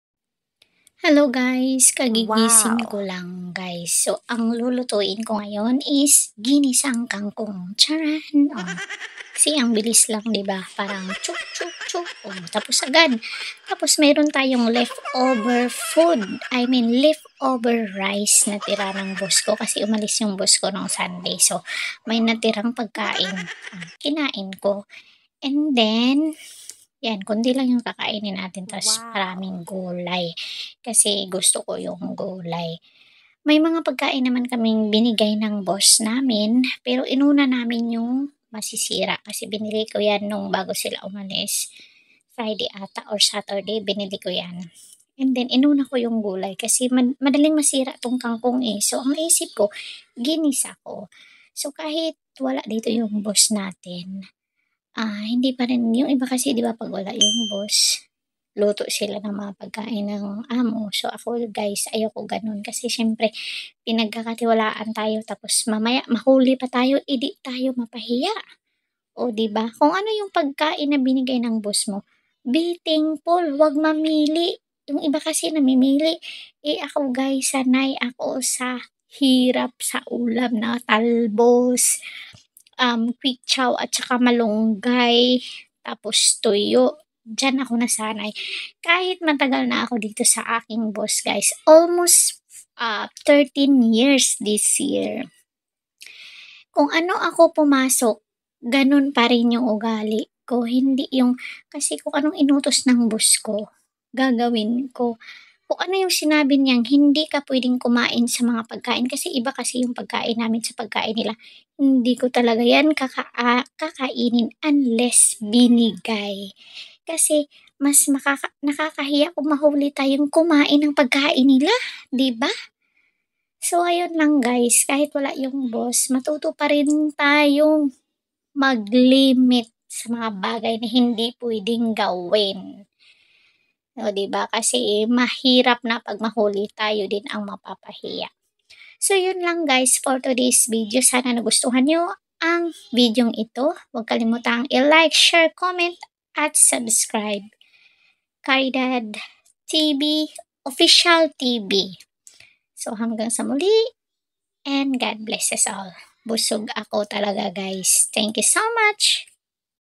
GRAVE🥵 si Inday Pag wala amo abusado Parang amo sa bahay ng amo😎
kasambahay dailyvlog bisayavlogger Ganito Karin ba Pag wala amo mo👈